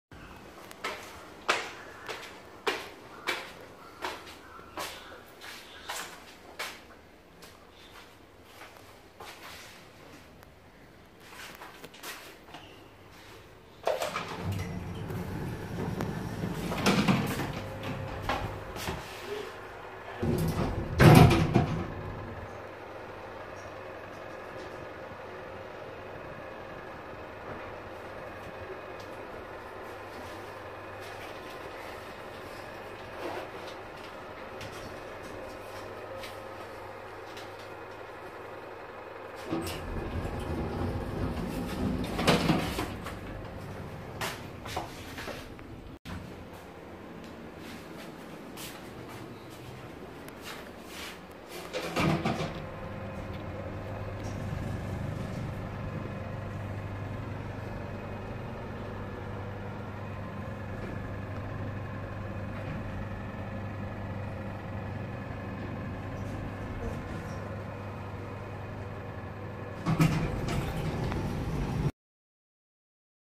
Ride on Blk 20 Redhill Close Fujitec Lift
Ro Noob_SBS2824C films Blk 20,A block equipped with Fujitec Lift
ft SBS2831G Transportations
Blocks that are locked
All Blocks Except for Blk 1 (As of 31/7/2019)
Update : This block is closed